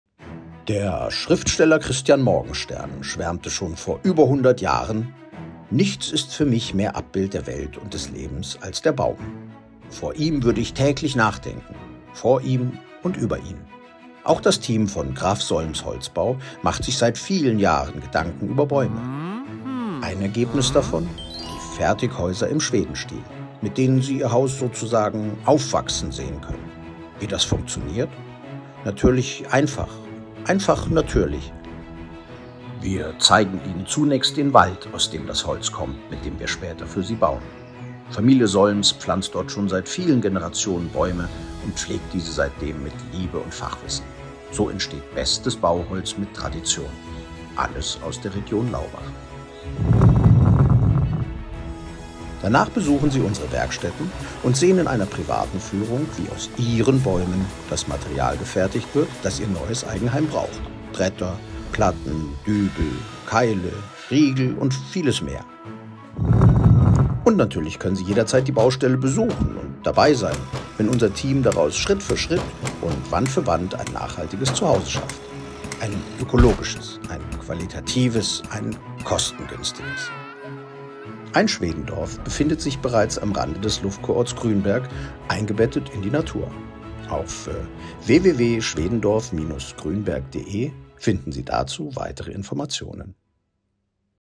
Das Schwedenhäuser-Erklärvideo
Karl Georg Graf zu Solms-Laubach hat mit seinen Schwedenhäusern eine nachhaltige Art zu bauen kultiviert. Gary Glotz hatte den Auftrag, das Konzept in einem kurzen Video potentiellen Kunden zu erklären.